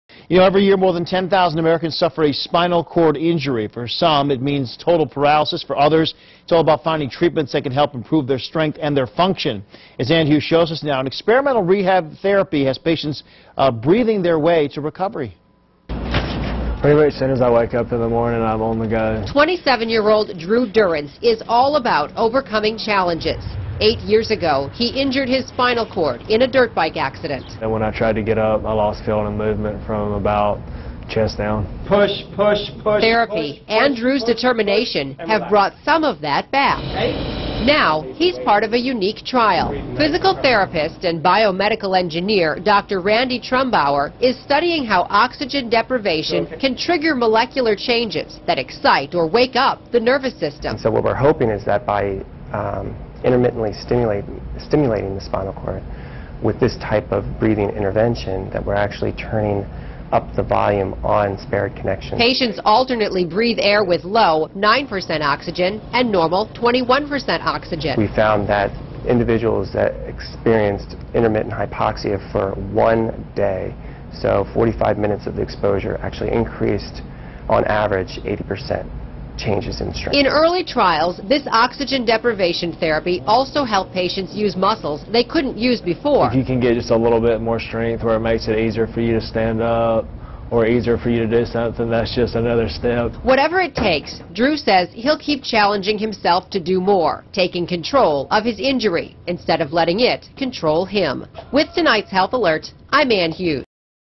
Intermittent Hypoxic Therapy (Altitude Training) for Spinal Cord Injury patients.
Emory University researchers are using high altitude training (low-oxygen equipment by Hypoxico) to stimulate nerve regeneration and rehabilitate Spinal Cord Injury Patients (SCI).